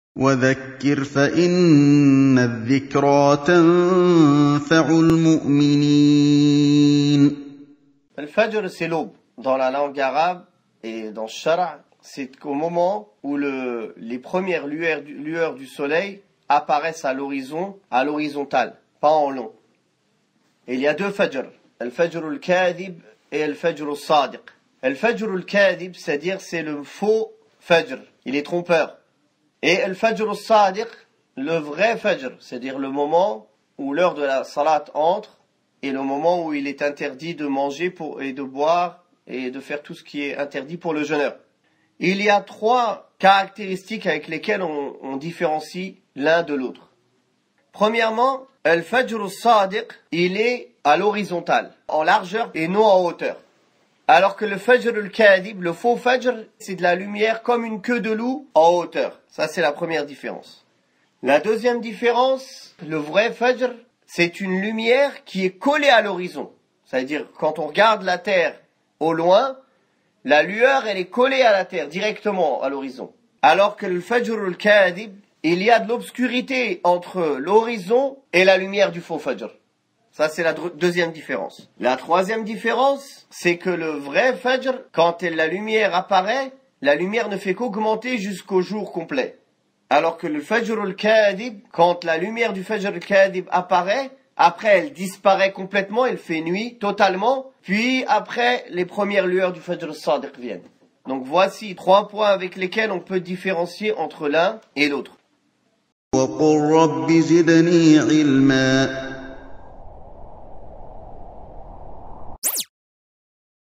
Les 3 differences du fajr SADIQ et KADHIB (Abou Laïth 'Othmân Al Armany) - Dourous-Sounnah.com
Extrait : Les 3 differences du fajr SADIQ et KADHIB.

Tafsir 028 Juzz 'Amma Al Fadjr (L'aube) (2013) Sourate 89